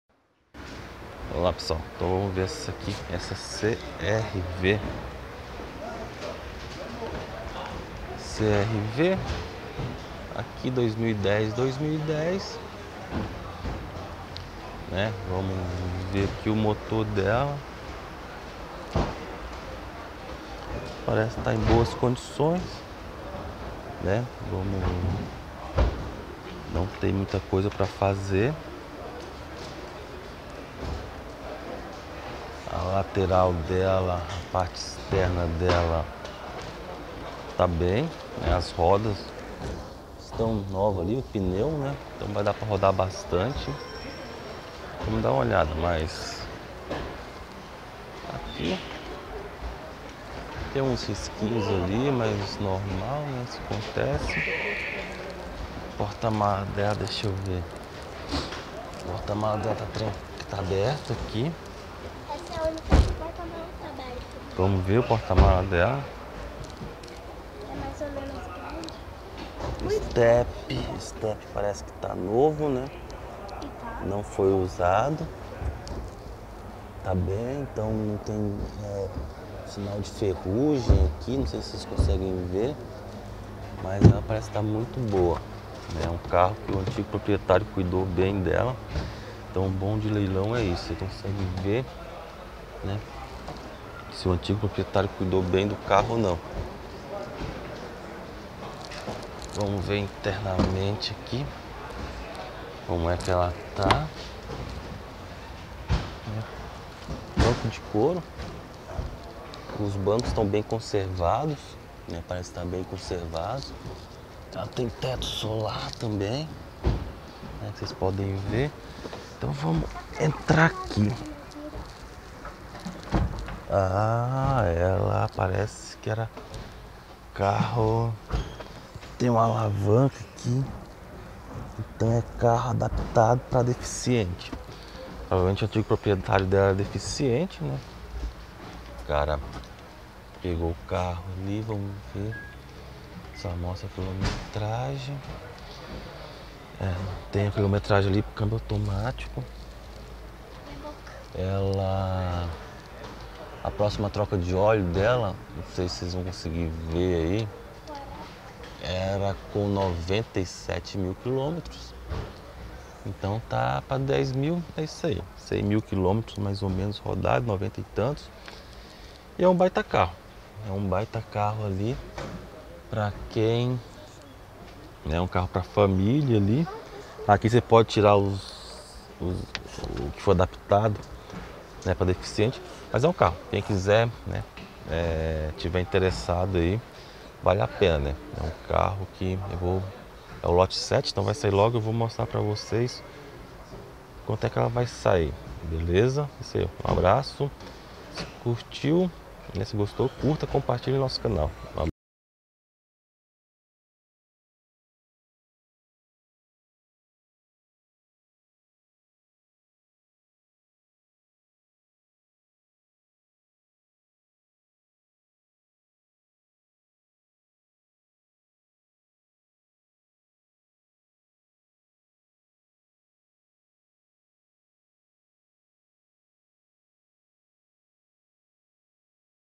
Leilão de CARROS, CR-V EXL 4X4 Cambio Automático
Veja essa  CR-V EXL 4X4 Cambio Automático Que encontrei em um leilão de carros.
Essa Honda CR-V EXL 4X4 Cambio Automático foi vendida leilão de caros por R$ 36.000.
Essa Mesma  CR-V EXL 4X4 Cambio Automático é vendida fora dos leilões de carros por R$ 51.000.